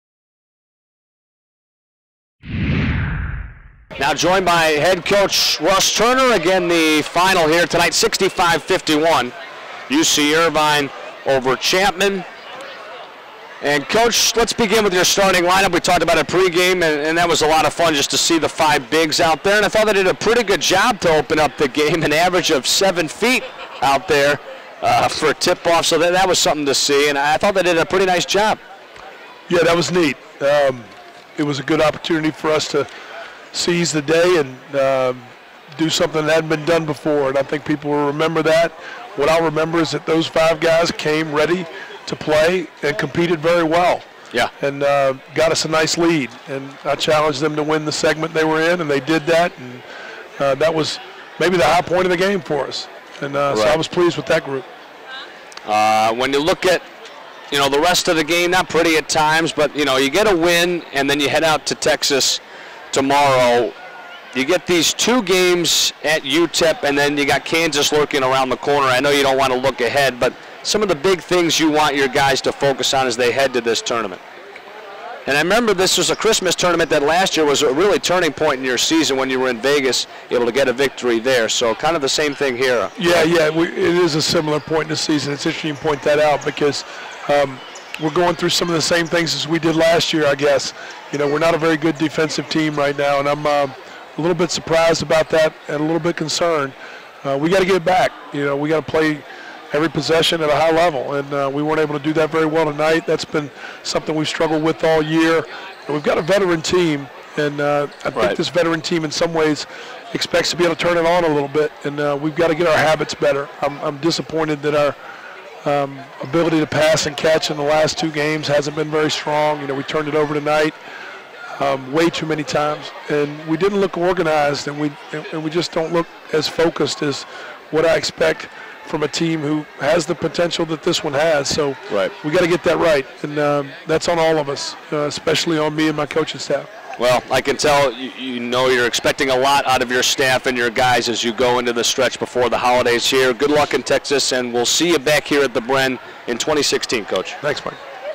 Men's Basketball vs. Chapman
The Anteaters improved to 8-4 on the season after a 65-51 win over Chapman.